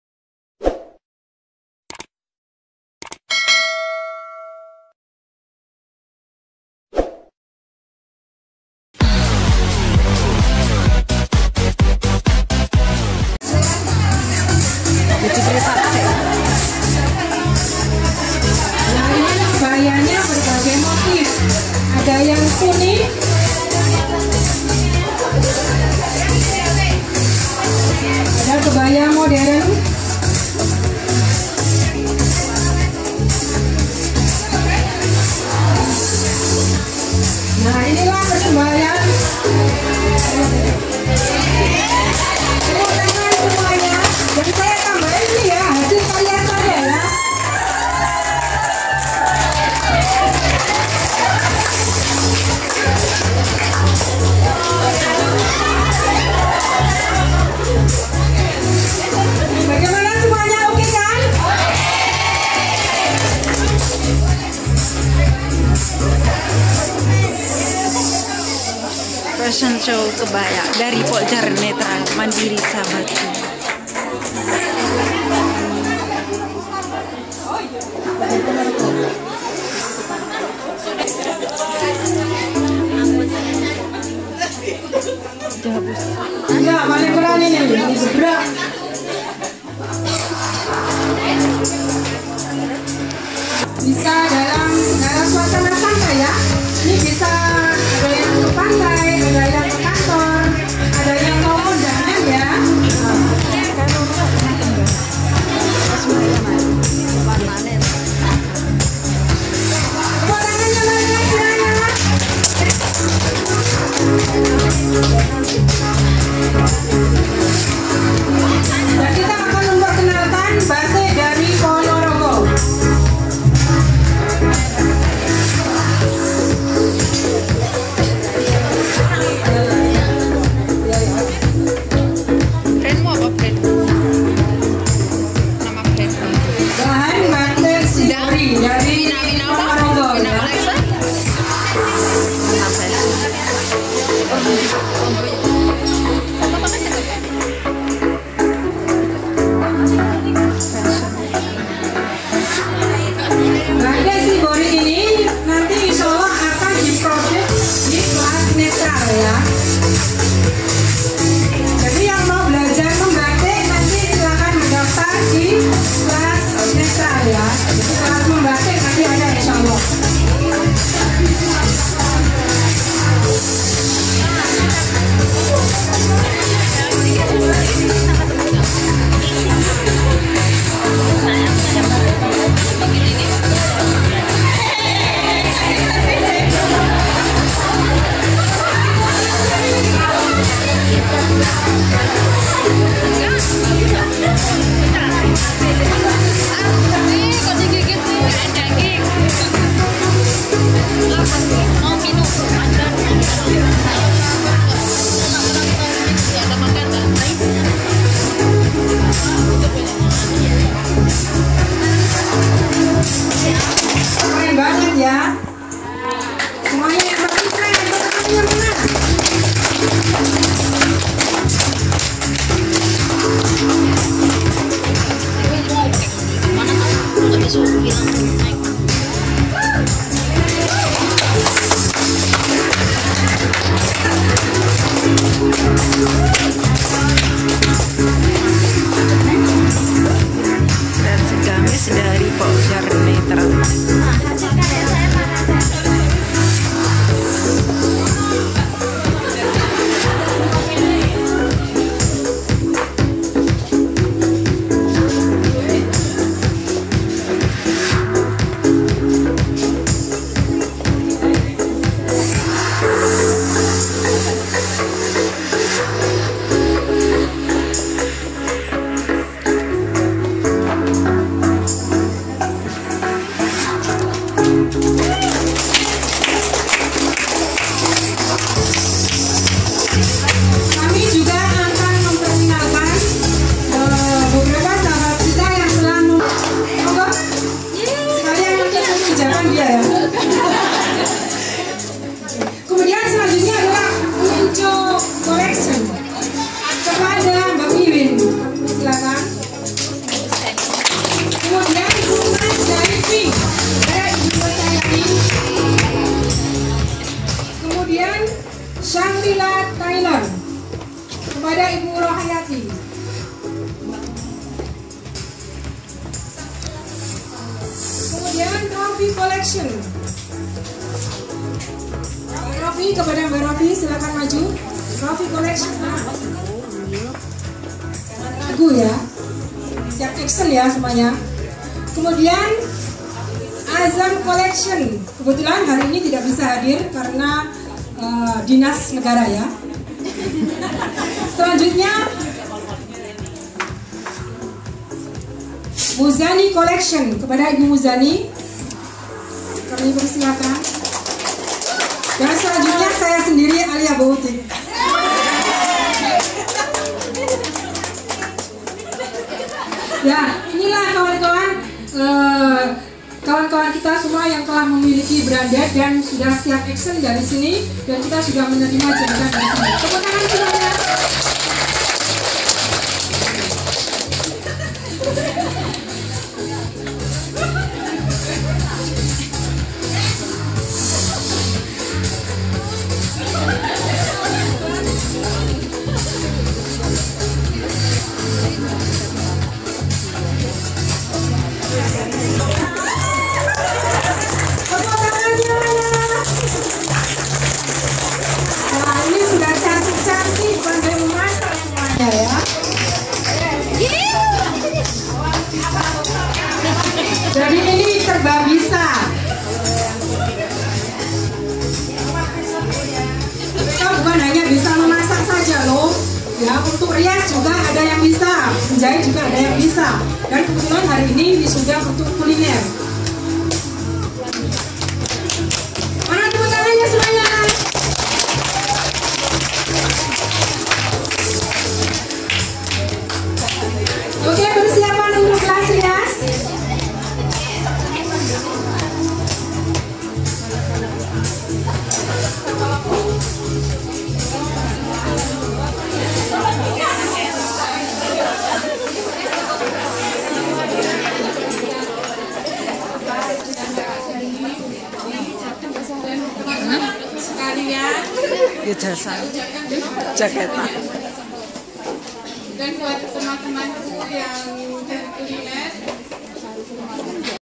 Acara fashion show wisudawati Netral #1
Acara fashion show anggota pokjar netral mandiri sahabatku,04082019